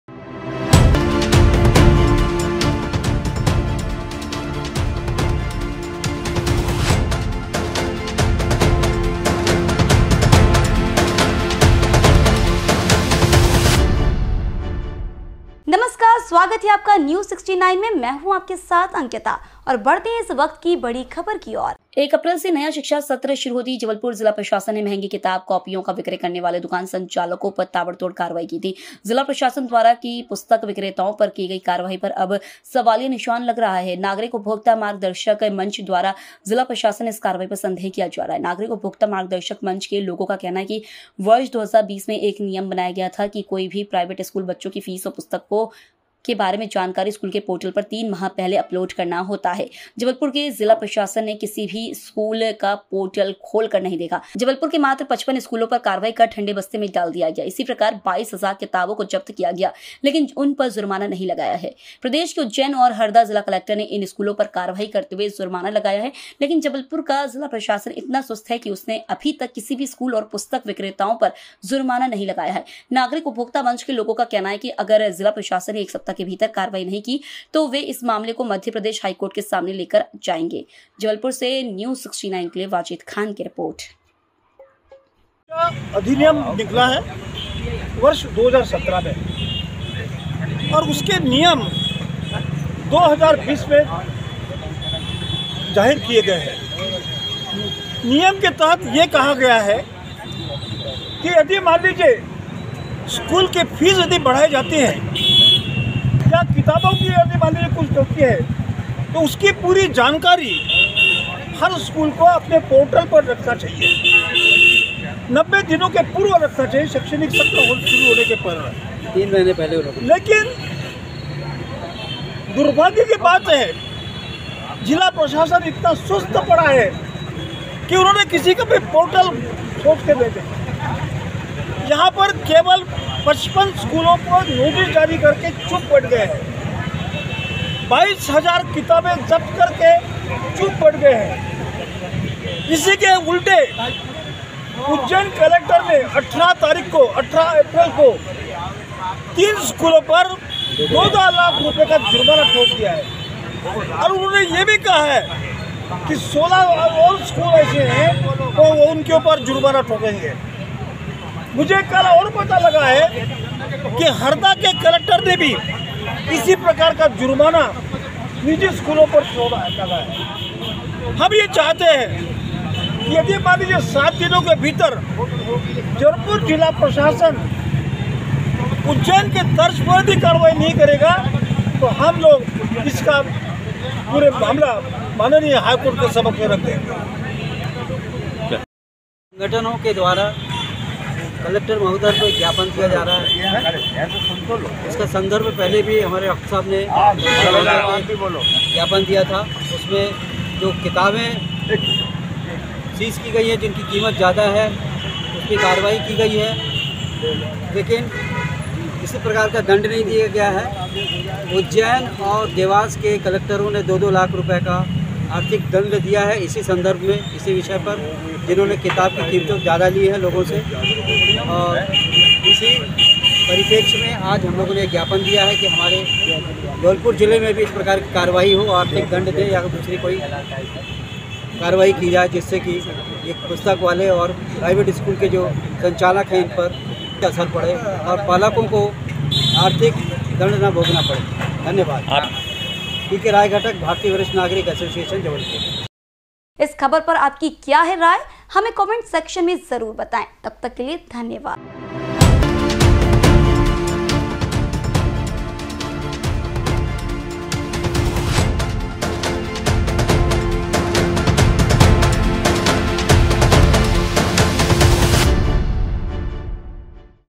निजी स्कूलों और पुस्तक विक्रेताओं पर कार्यवाही न होने पर नागरिक उपभोक्ता मंच की चेतावनी।
जिला प्रशासन द्वारा निजी स्कूलों और पुस्तक विक्रेताओं पर कार्यवाही न होने पर नागरिक उपभोक्ता मंच की चेतावनी।